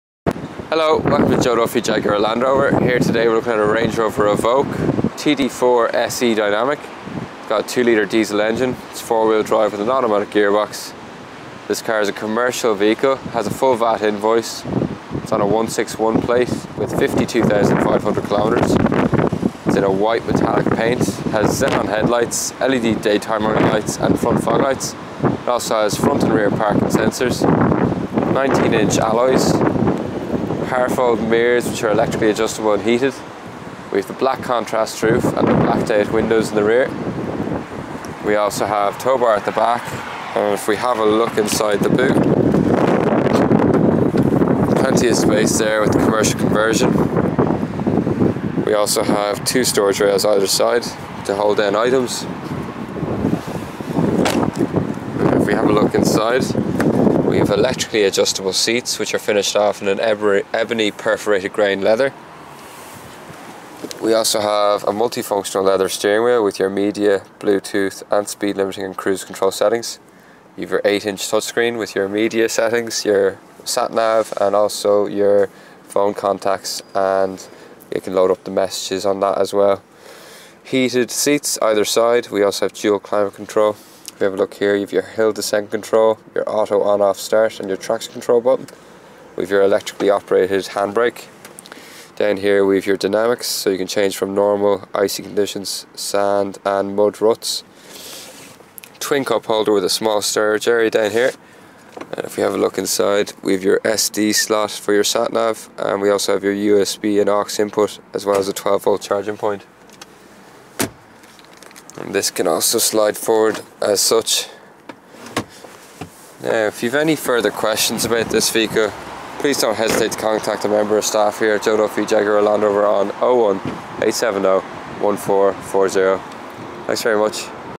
161MH2939 - 2016 Land Rover Range Rover Evoque TD4 SE Dynamic Commercial Fu...
Here is a video of our 161MH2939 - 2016 Land Rover Range Rover Evoque TD4 SE Dynamic Commercial Full VAT Invoice 39,995. Please contact Sales to arrange a test drive today.  Joe Duffy Land Roverhttp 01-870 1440